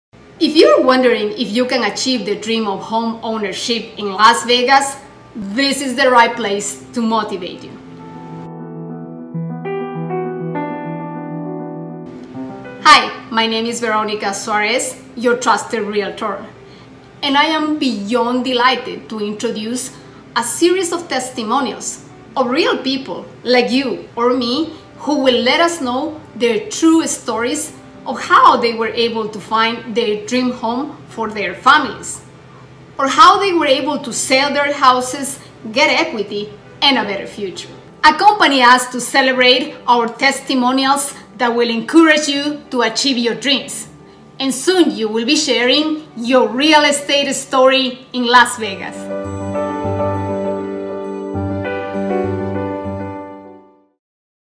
MY REAL ESTATE STORY IN LAS VEGAS
If you are wondering if you can achieve the Dream of Homeownership 🏠 in Las Vegas, this is the right video to encourage you!! Accompany us to celebrate our testimonials that will motivate you to accomplish your dreams, and soon you'll be sharing your Real Estate Story in Las Vegas.